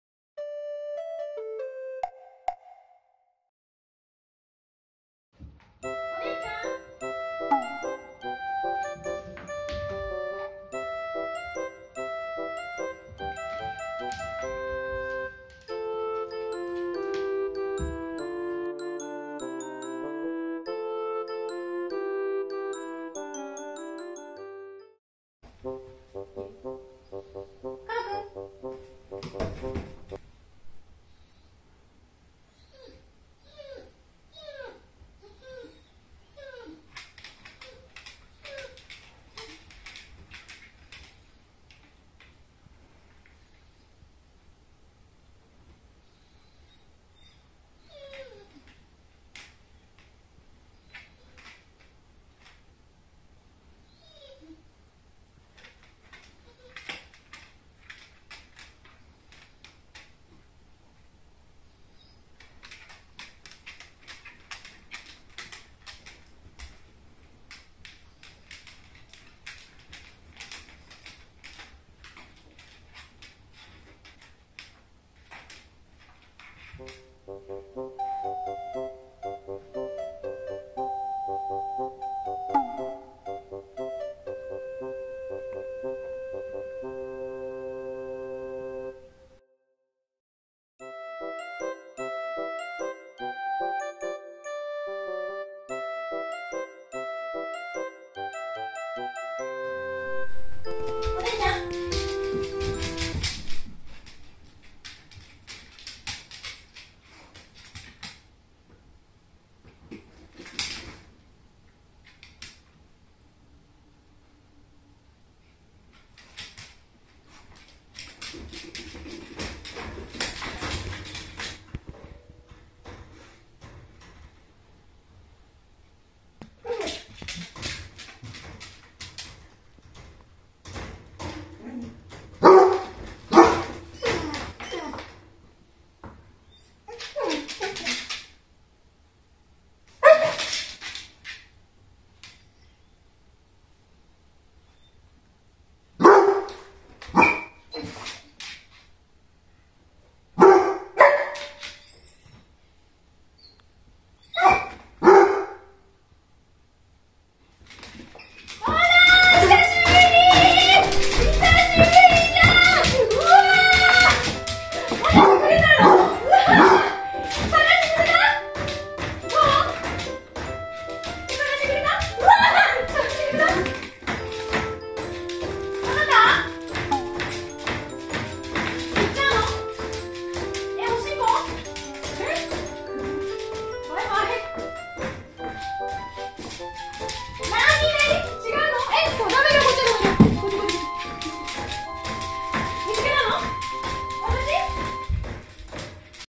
また急に飼い主が消えてみた
２年前にやったブランケットトリックネタです
初めて見るカバくんの反応は…？
２回目のおでんの反応はいかに…？